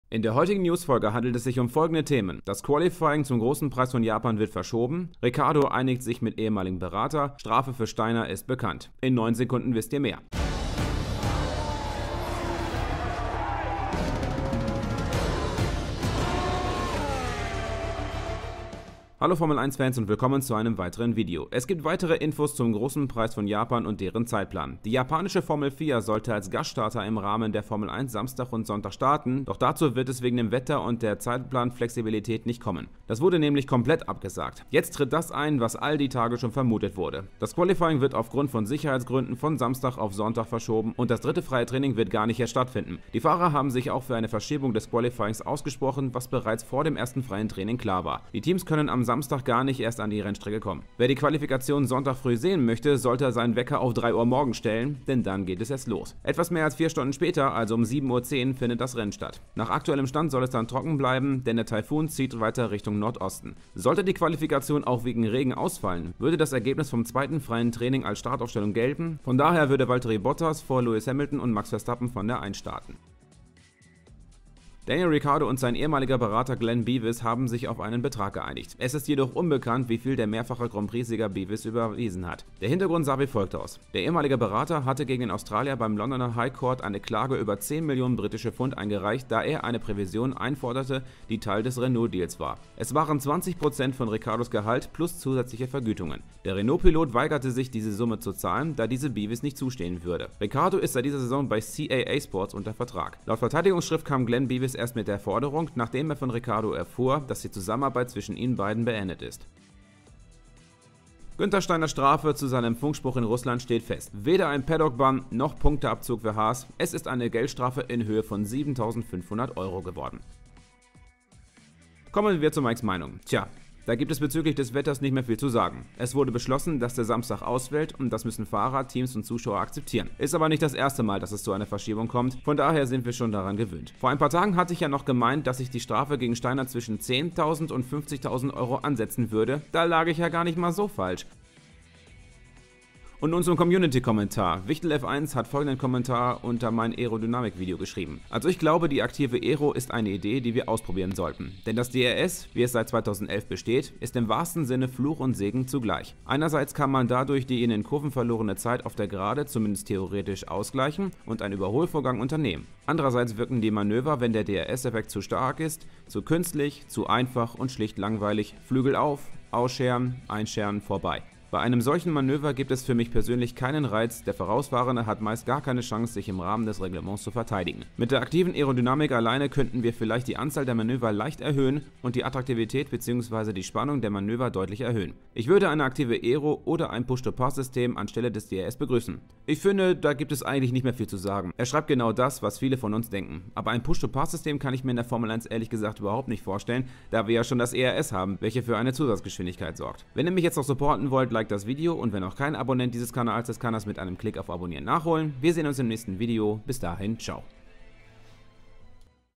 Das Qualifying zum Japan GP wird verschoben | F1 News | Maik's F1 Channel
Es gibt weitere Infos zum GP und dem Zeitplan. Die japanische Formel 4 sollte als Gaststarter im Rahmen der F1 Samstag und Sonntag starten, doch dazu wird es wegen dem Wetter und der Zeitplan-Flexibilität nicht kommen. Das wurde nämlich komplett abgesagt. Weitere Infos im Video.

🖥 🎤 🖱️ MEINE AUSRÜSTUNG
Mein Hauptmonitor
Mein 2. Monitor